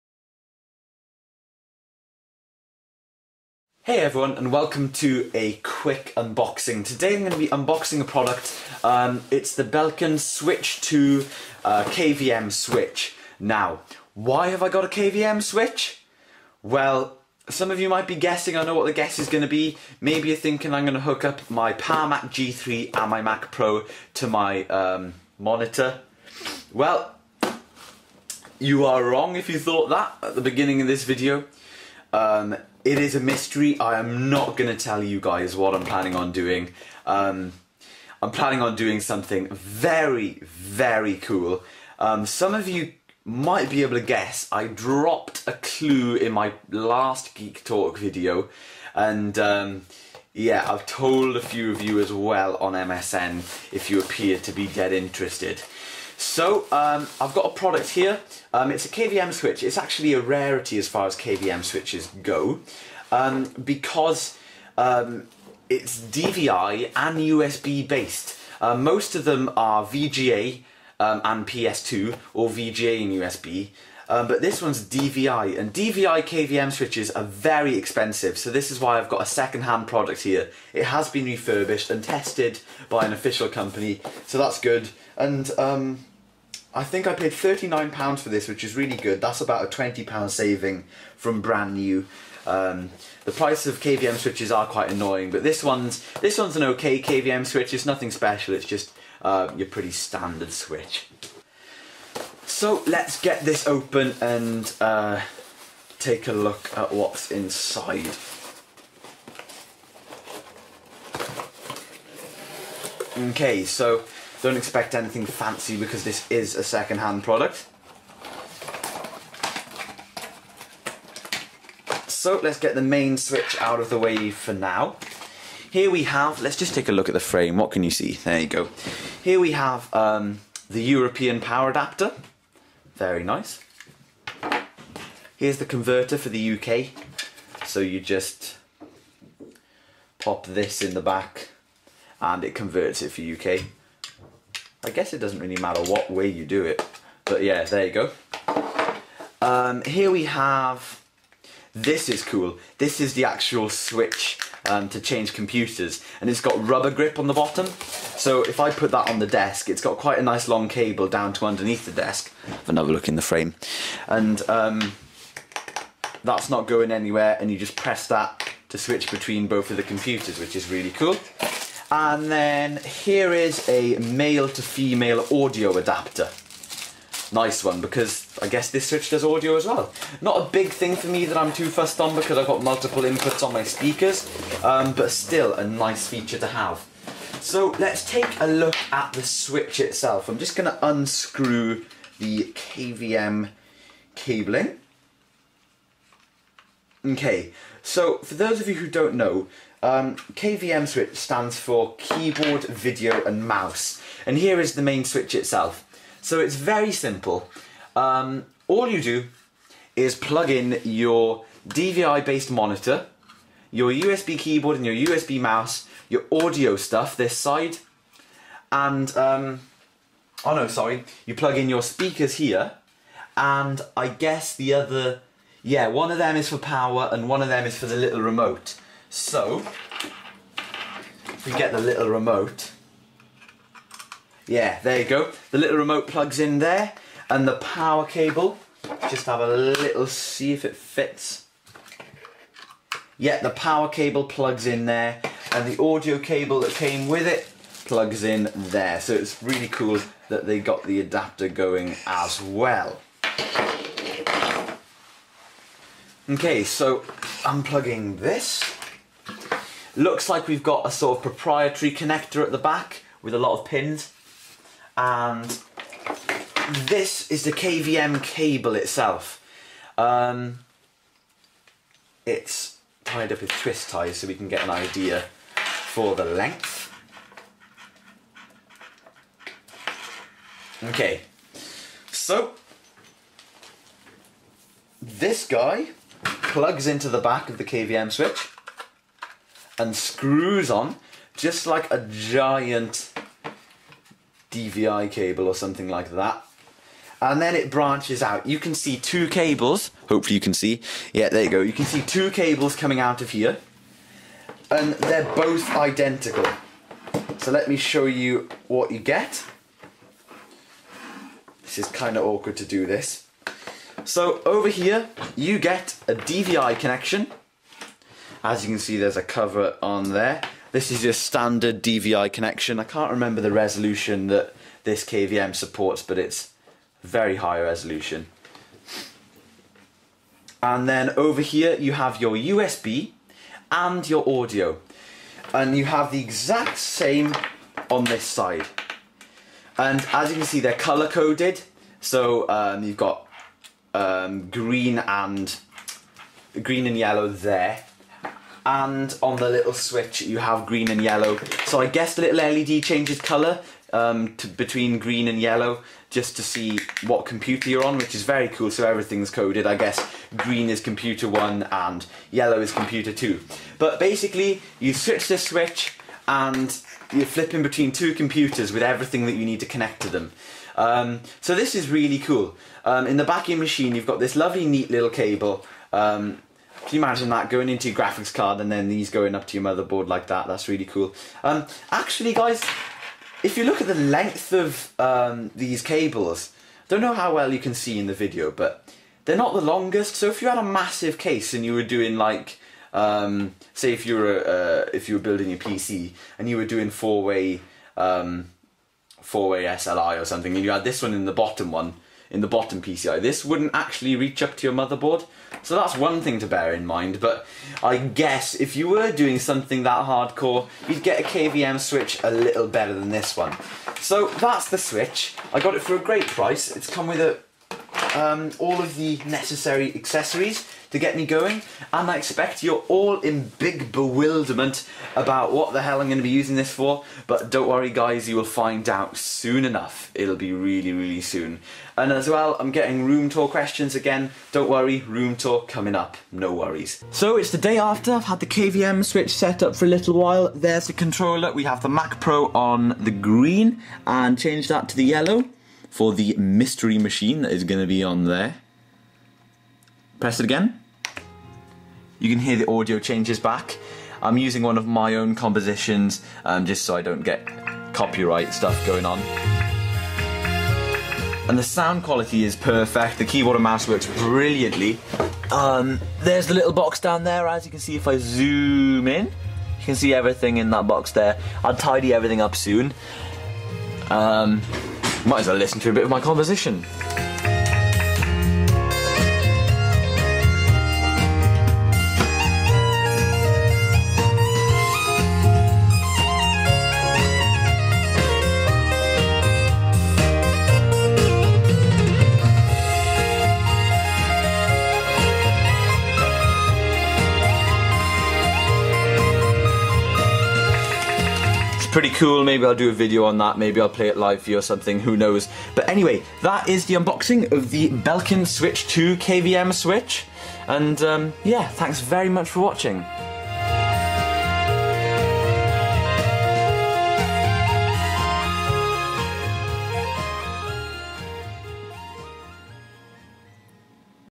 Unboxing & Overview: Belkin Switch2 KVM Switch
A simple Unboxing and Overview of a very nice USB/DVI KVM switch. I wonder what I'm going to use this for?